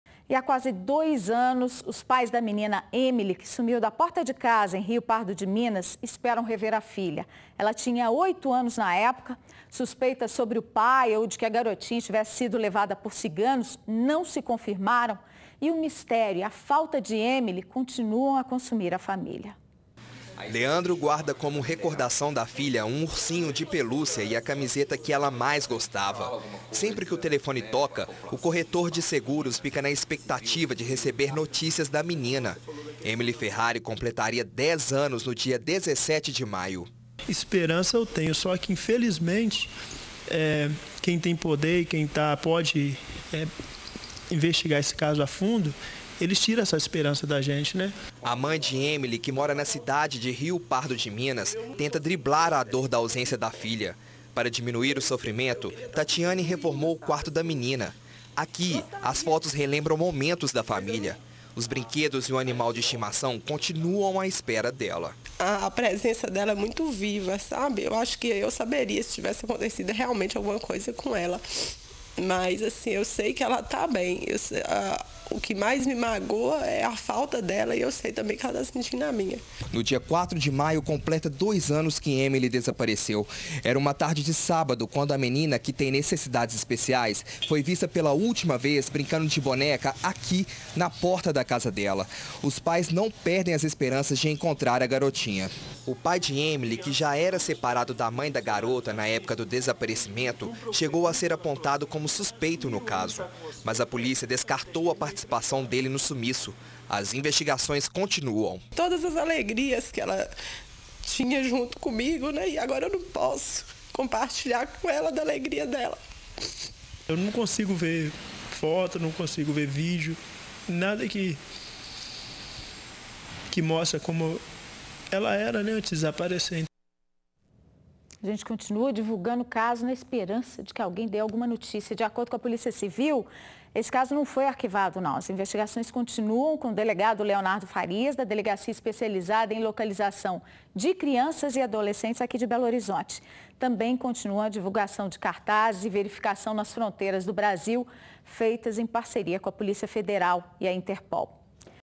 Dois anos após desaparecimento, família da menina Emily espera solução para caso
Há quase dois anos, os pais da menina Emily, que sumiu da porta de casa em rio pardo de minas, esperam rever a filha. Ela tinha oito anos. Suspeitas sobre o pai ou de que a garotinha tivesse sido levada por ciganos não se confirmaram. E o mistério e a falta de Emily continuam a consumir a família.